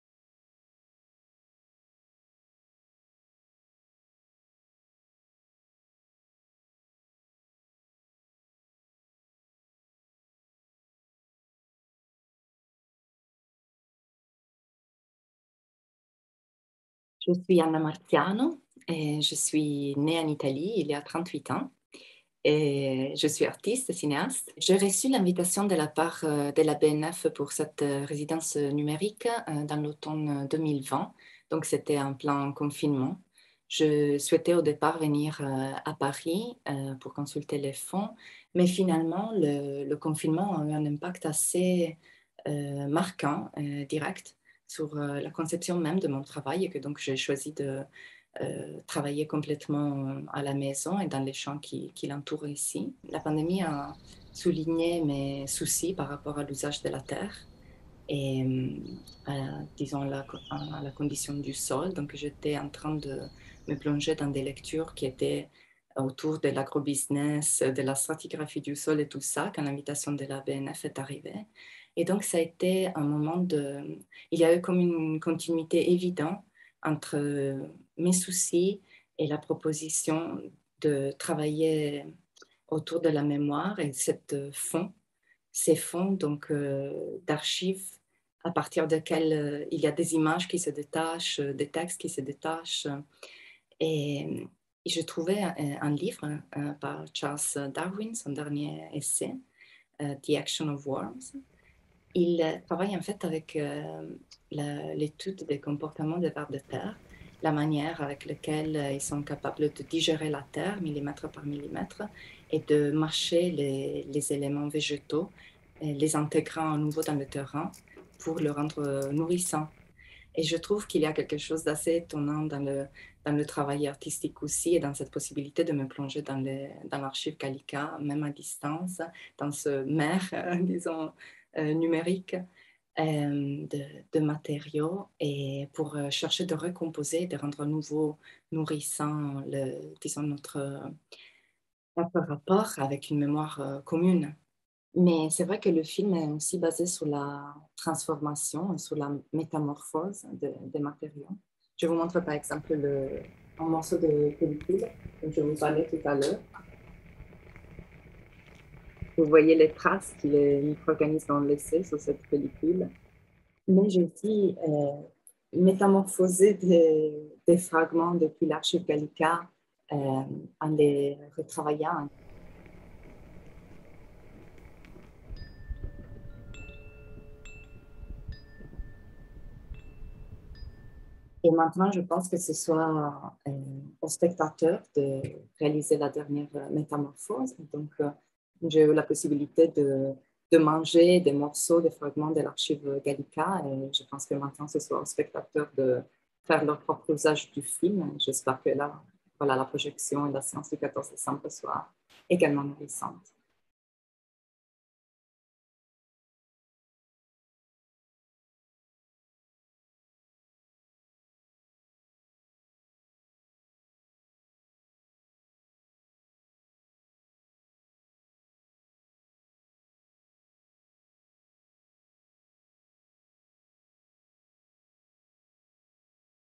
Rencontre avec Anna Marziano, artiste en résidence numérique
L’artiste vidéaste Anna Marziano, lauréate 2020 de la résidence numérique BnF-Fondation Simone et Cino Del Duca, présente le fruit de son travail de création, à partir des fonds de Gallica, la bibliothèque numérique de la BnF, le 14 décembre 2021.

Fragments de textes, estampes et films nourrissent le projet d’Anna Marziano depuis l’hiver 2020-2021. Elle s’est également inspirée de l’action de transformation et de régénération du sol, faisant là un parallèle avec sa propre exploration des collections patrimoniales.

Qui est Anna Marziano ?
Anna Marziano, née en Italie en 1982, est réalisatrice, directrice de la photo et ingénieure du son. Sa pratique consiste à expérimenter avec la forme documentaire, afin d’explorer la construction des identités contemporaines par un procédé filmique qui questionne la subjectivité et les rôles sociaux. Elle porte une grande attention à la forme aussi bien qu’au processus du film et à son éthique. Ses films sont projetés dans des festivals et des espaces artistiques dans le monde entier. Elle a reçu en 2018 pour Beyond the one le prix A. Mekas au festival Experimenta, ainsi qu’une mention spéciale au Cinéma du Réel, et en 2020, pour Al largo, le prix spécial du jury au Festival du film de Turin.